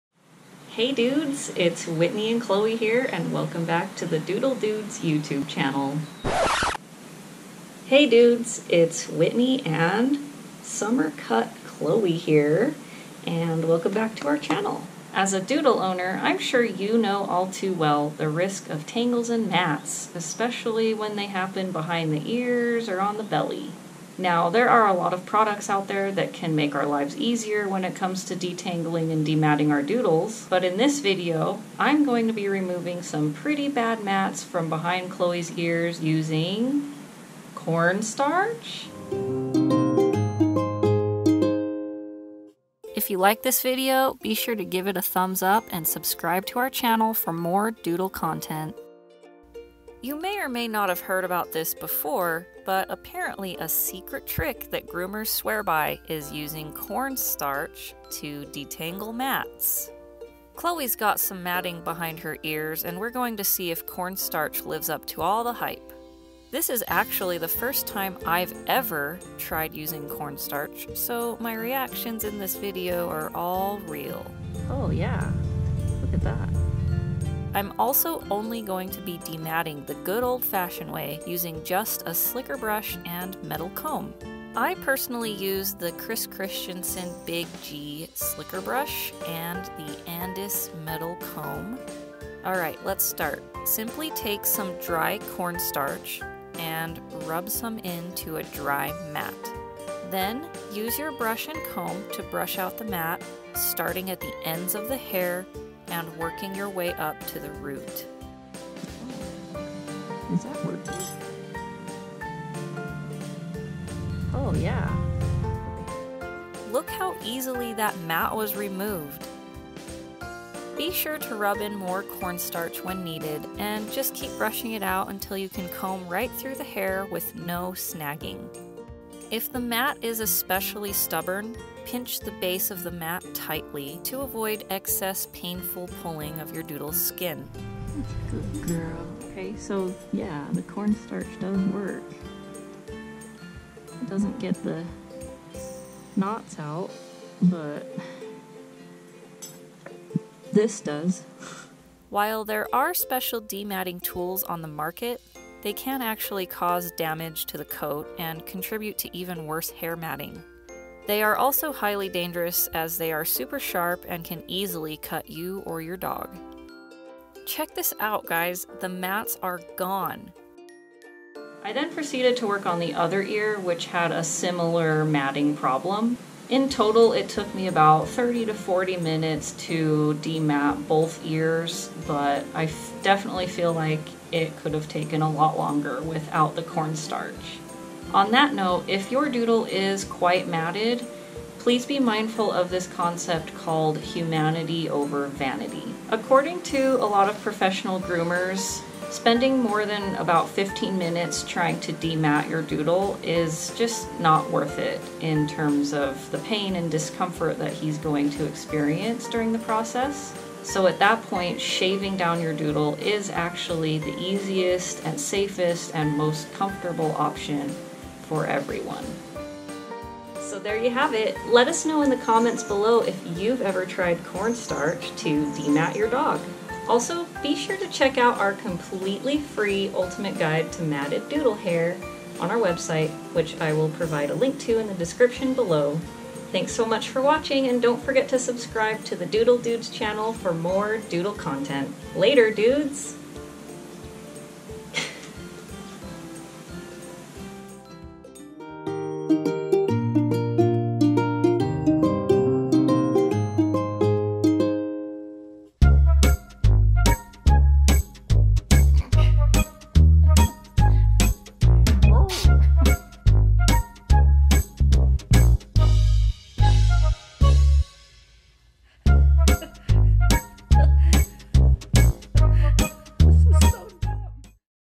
How to Remove Matted Doodle Dog Hair Behind the Ears With Cornstarch, Slicker Brush & Metal Comb
Watch as we remove badly matted dog hair behind the ears a Goldendoodle with cornstarch. Is it effective?